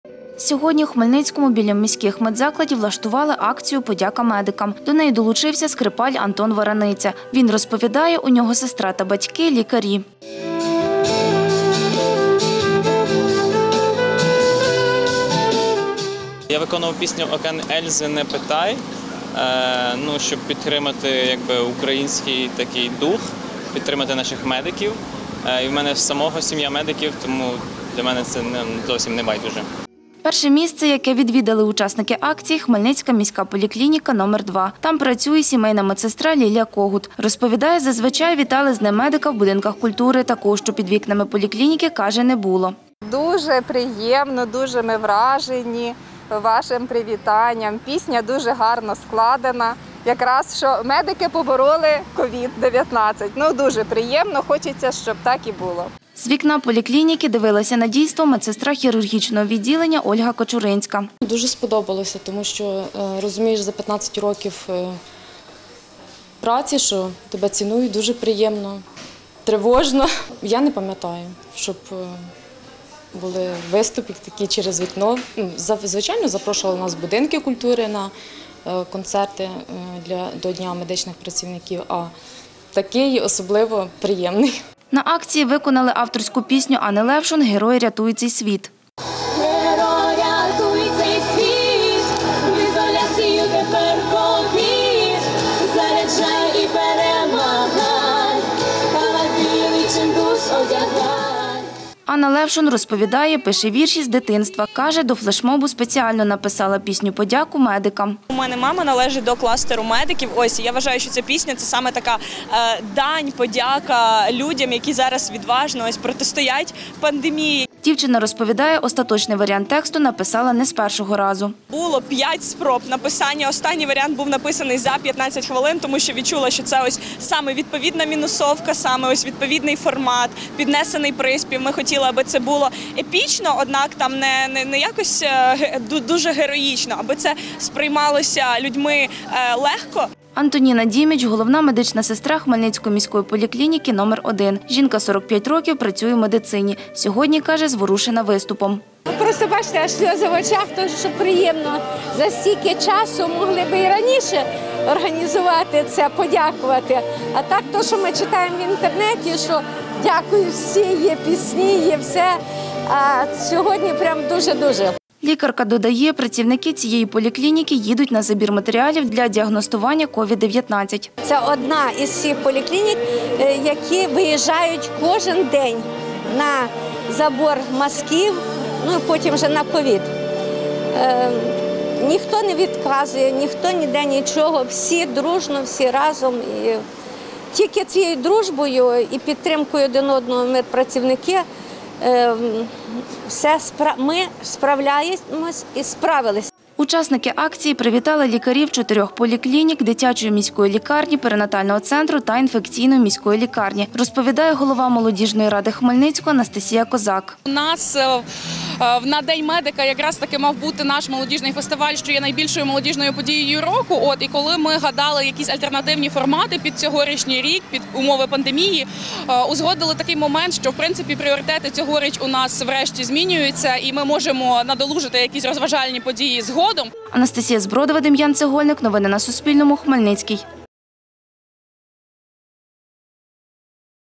Піснями привітали лікарів Хмельницького, напередодні Дня медика молодь міста.
Піснями привітали лікарів Хмельницького, напередодні Дня медика молодь міста. Виконували інструментальну композицію, дві українські пісні та авторську пісню-подяку. Про це Суспільному сказала голова Молодіжної ради Хмельницького та організаторка акції Анастасія Козак. За її словами об’їхали усі стаціонарні медзаклади міста, де близько 20 хвилин проводили флешмоб. Анастасія Козак додає, наступного тижня планують зробити офіційне відкриття тематичного муралу на боковому фасаді дитячої міської лікарні